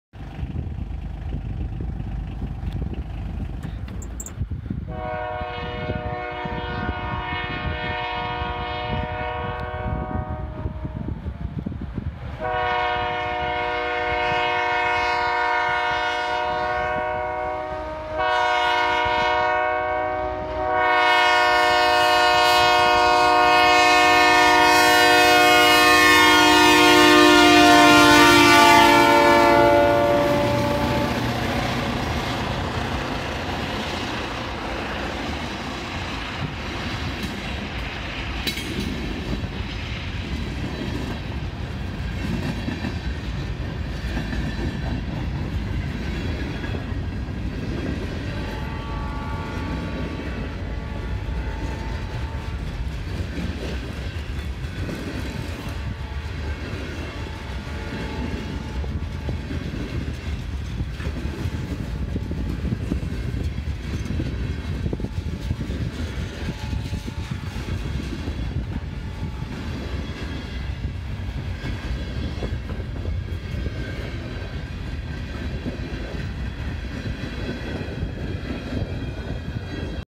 Decatur & Eastern Illinois Snow Train
On January 13, 2019 the Decatur & Eastern Illinois RR ran an eastbound train out of Decatur after a night of strong winds blowing around the few inches of snow that fell the previous day. Video finds HLCX 4212 and FURX 5551 leading a 29 car train through Long Creek, just outside of Decatur, IL. WATCO recently acquired this line from CSX and has quickly been growing traffic levels.